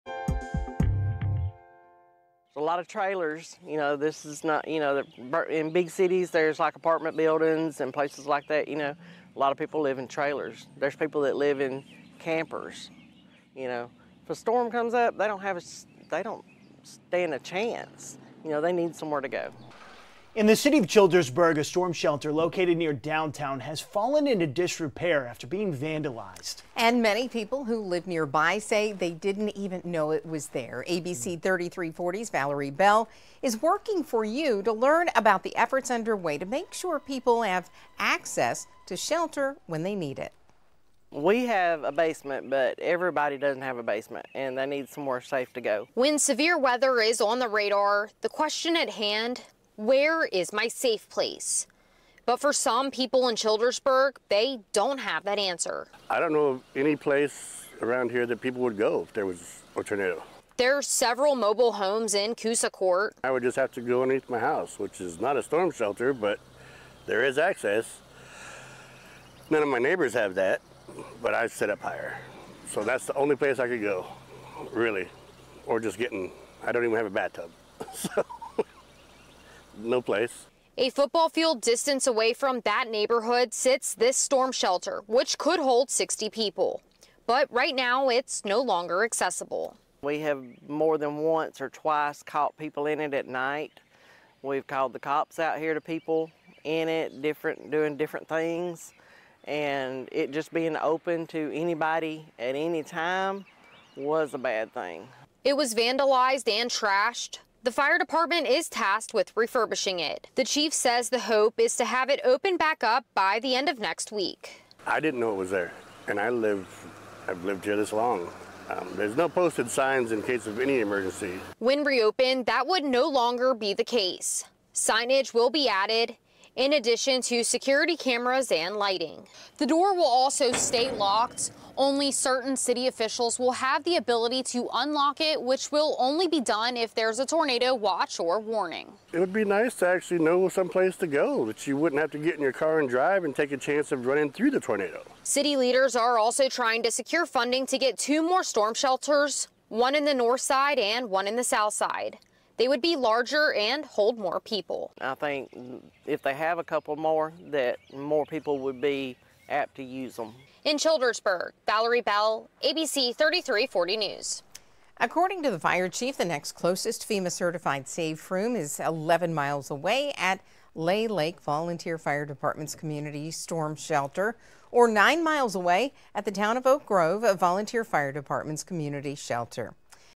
Childersburg refurbishing storm shelter after vandalism, seeking funding for more shelters
There are efforts underway in Childersburg to refurbish the city’s storm shelter for it to be in a condition that is safe for use. Additionally, the city is making efforts to try and secure federal funding to get two storm shelters. More